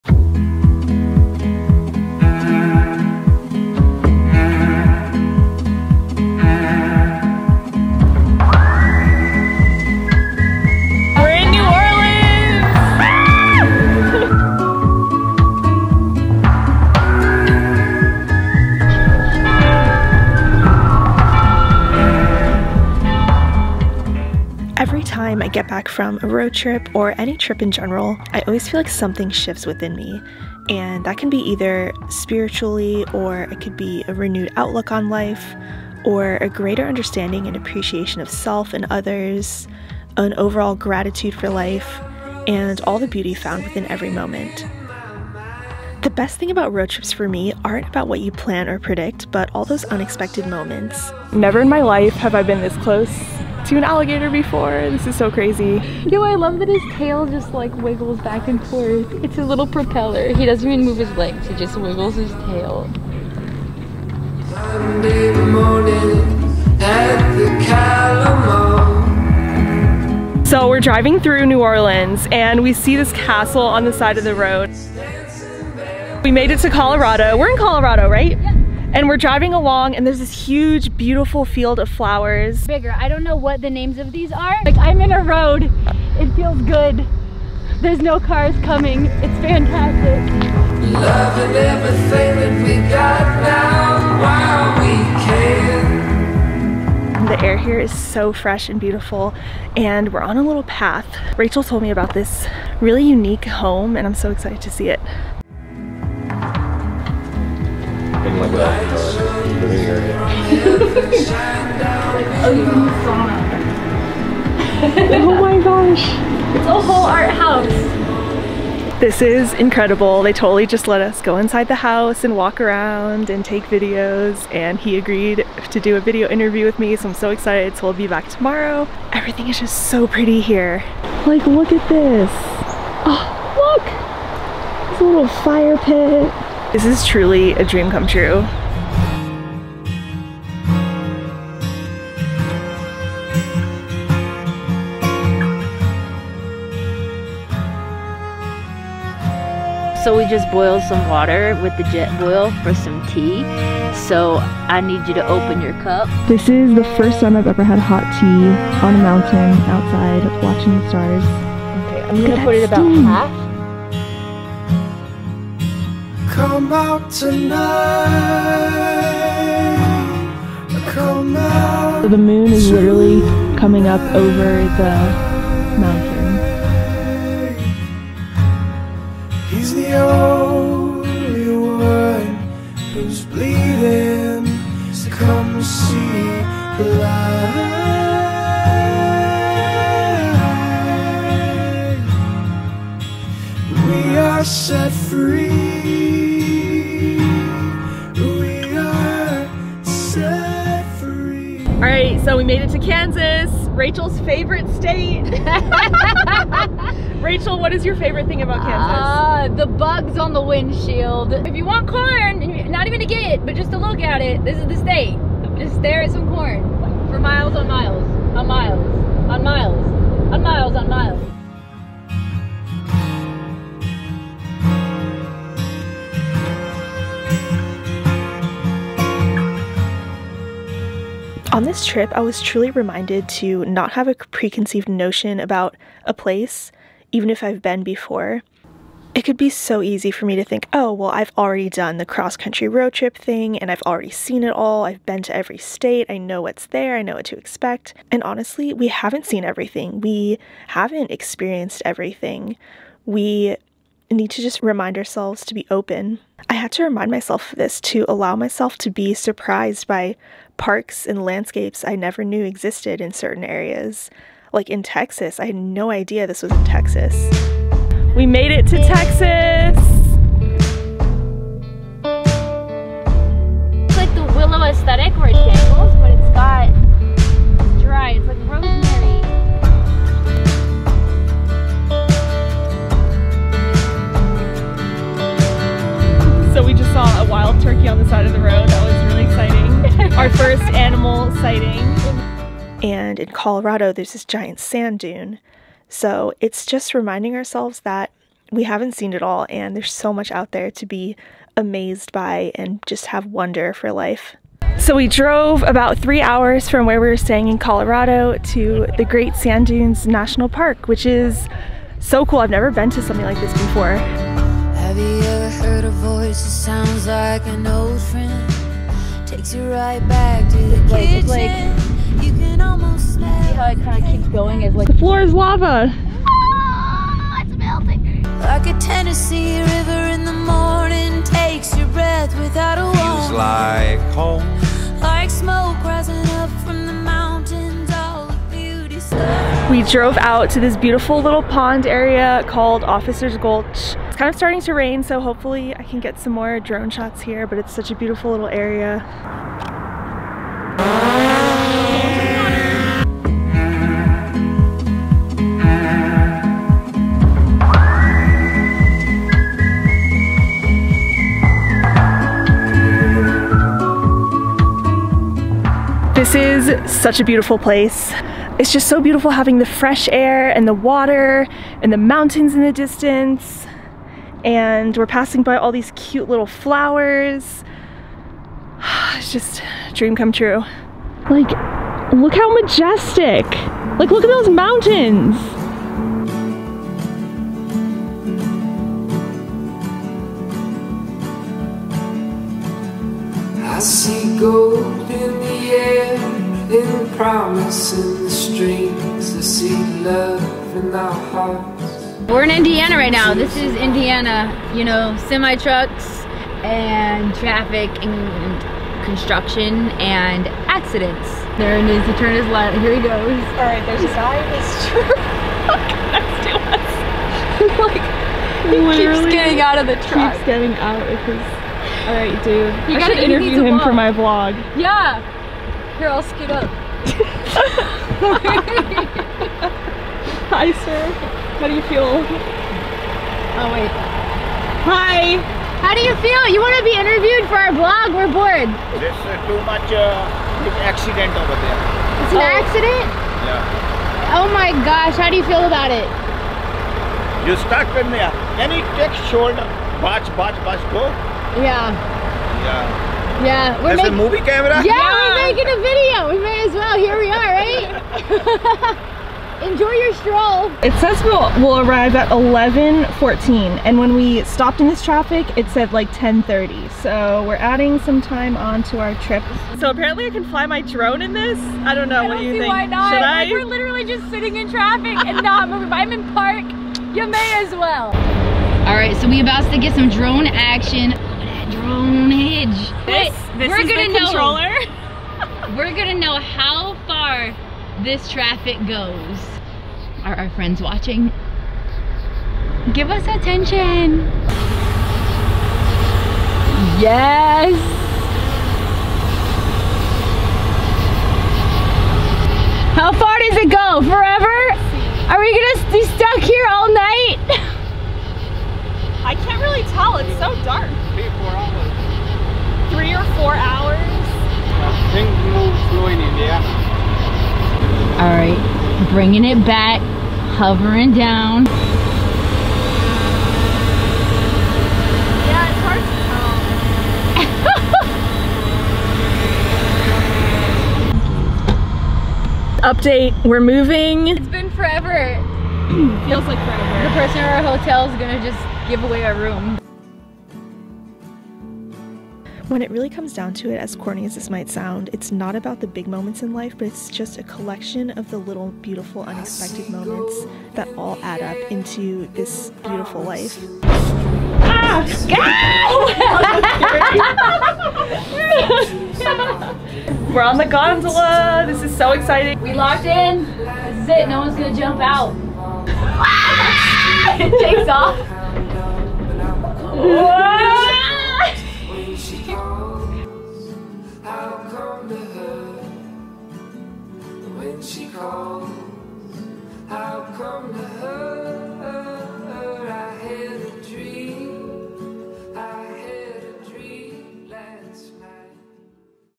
Road Trip Diaries | My Road Trip Experience (FL to CO)
As summer comes to an end, I am so grateful for the experiences and beauty I experienced along the way. I wanted to share some of the lessons I've learned on this road trip. I highly encourage everyone at some point to plan a getaway for these incredible views and amazing experiences.

During this road trip the places we visited (shown in this video) were:
○  Big Cypress National Preserve Florida
○  New Orleans
○  Colorado (Silverthorne, Officers Gulch, Loveland Pass, Great Sand Dunes, Breckenridge Gondola)
○  Texas (Palo Duro Canyon State Park)
○  Kansas
○  Indiana

My gear:
Camera: Sony a7 SIII
Lens: Sony 16-35 GM F2.8

🎥 Full list of my gear

📚 My Favorite Books on Creativity